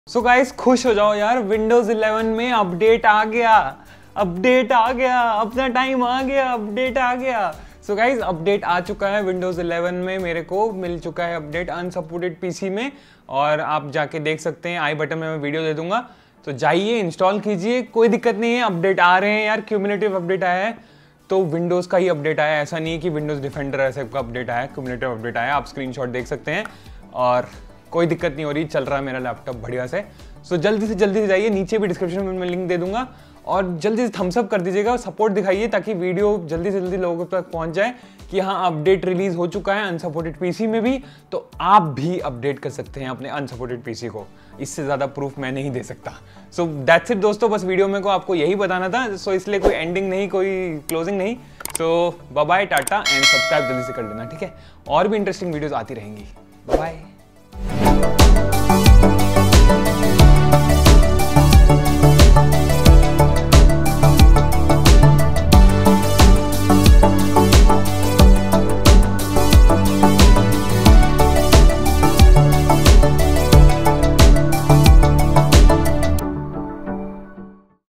Windows 11 Updates on Unsupported PCs | How to get Updates ?
In this video I am going to tell you how to get Windows 11 Updates on unsupported PCs and how to fix it. Watch the step by step guide and enjoy Windows 11 for FREE.

Amazon US

Amazon Business (Buy things for your Business at a discount)